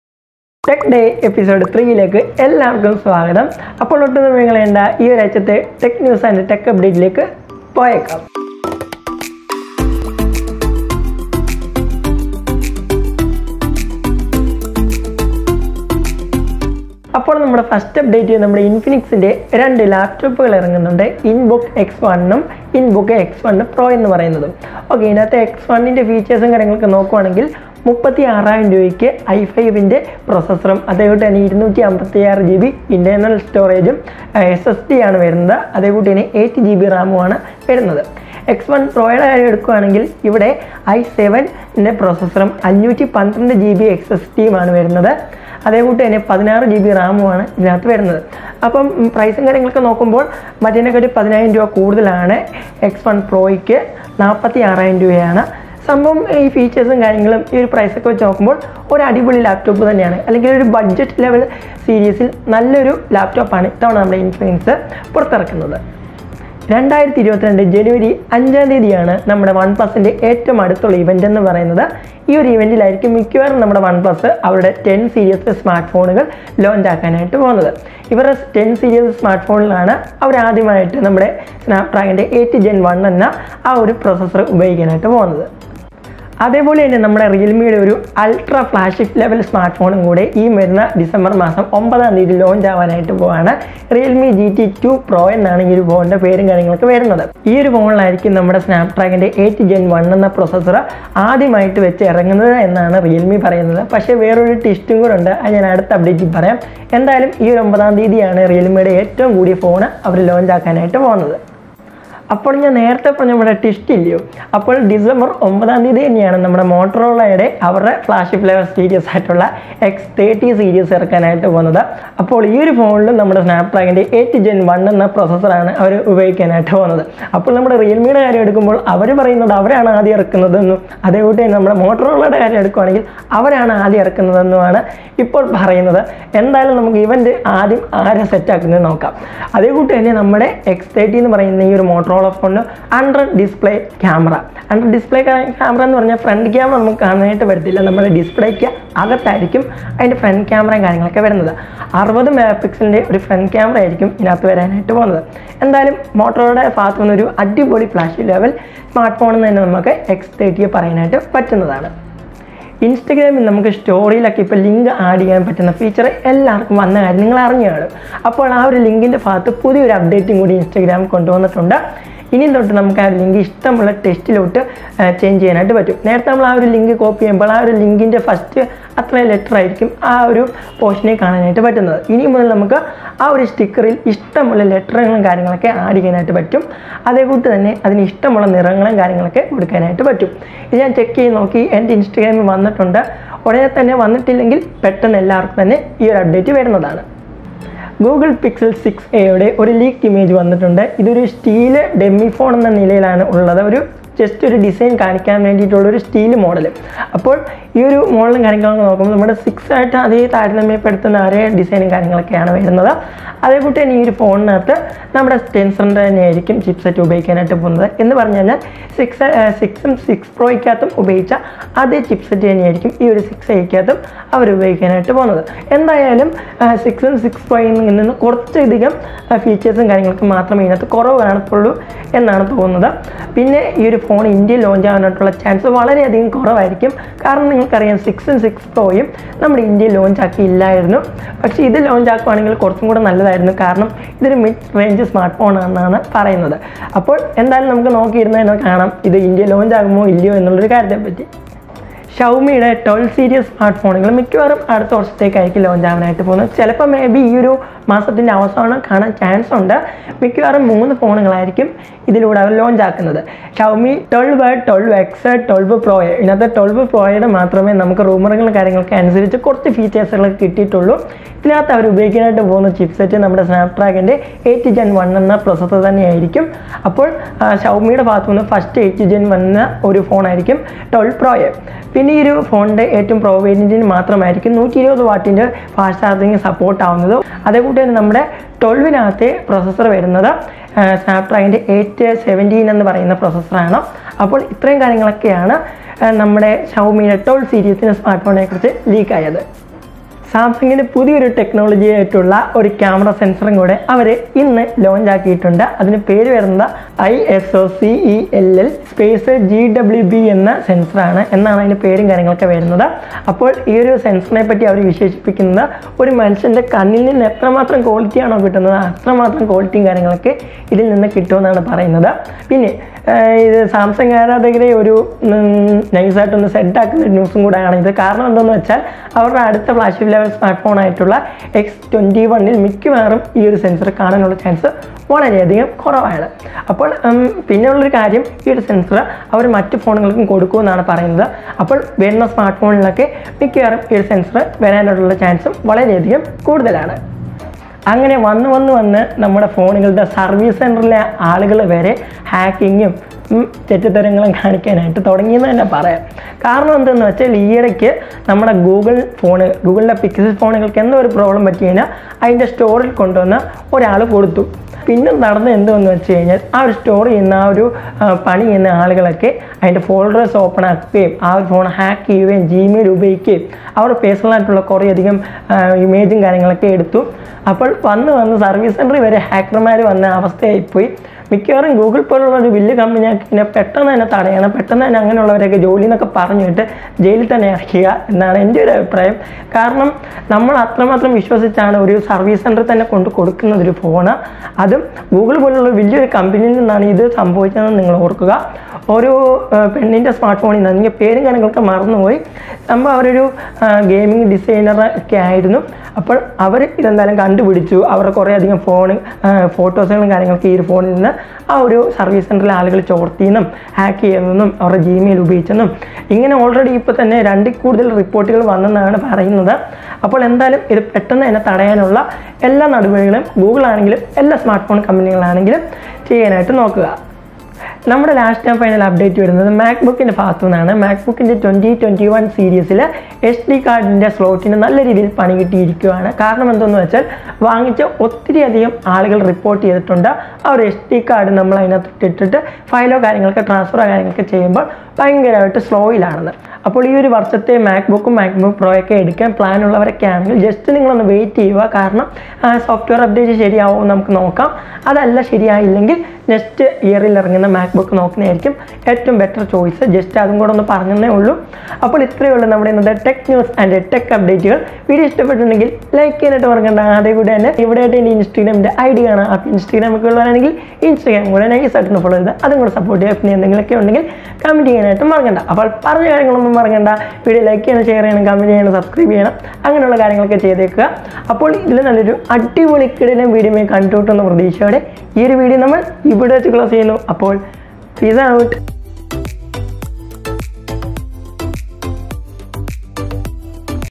Tech Day #3 | Realme GT 2 Pro, Infinix laptop, One Plus 10, Google Pixel 6a, Xiaomi 12, Motorola X30
Hallo Friends